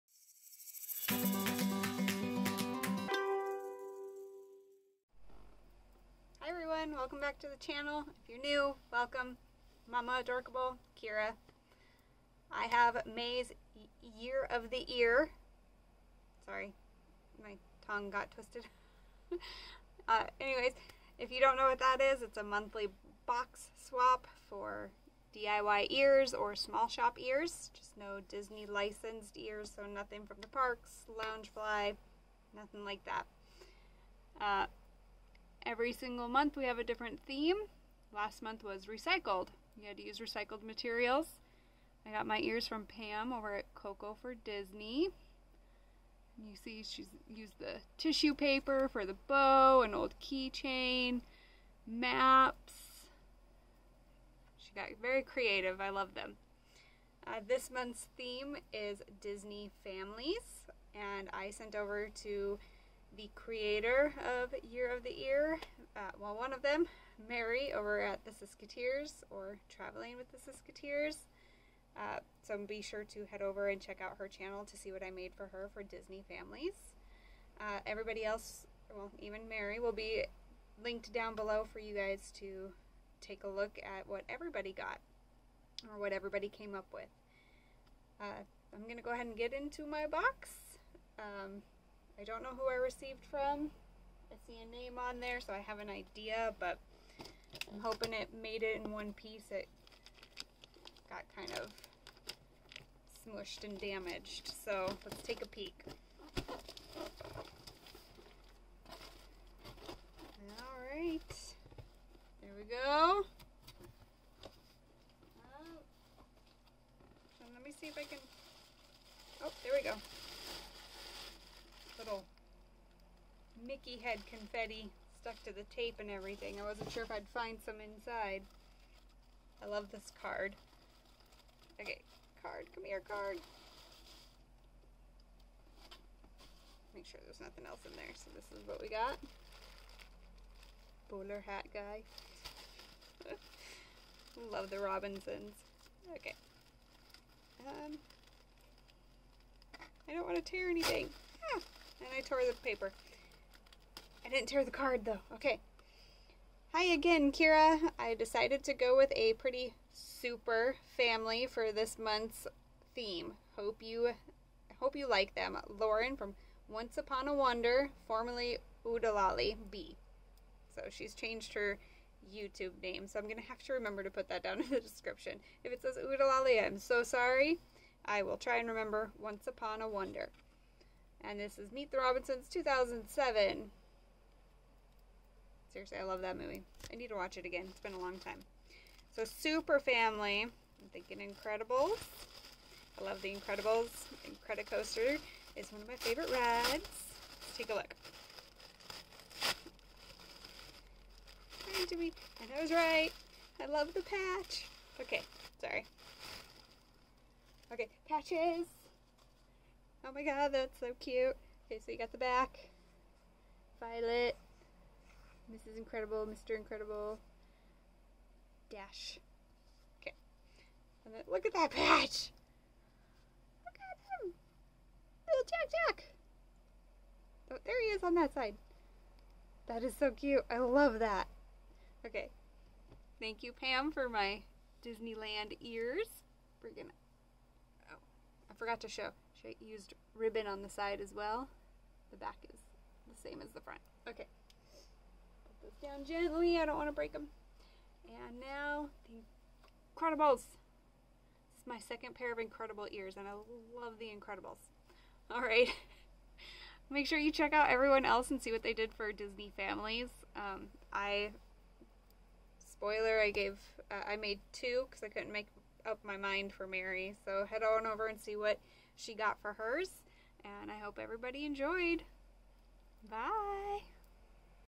Year of the Ear - May 2024
Hello Hello Hello :-D
Welcome to May's Year of the Ear. Our theme for this month is Disney Family/Families. I am super excited to see what families everyone decided to represent in their ears!! As always, please hop over to everyone's channels listed below to see what everyone got.

Be sure to follow on Instagram too :-)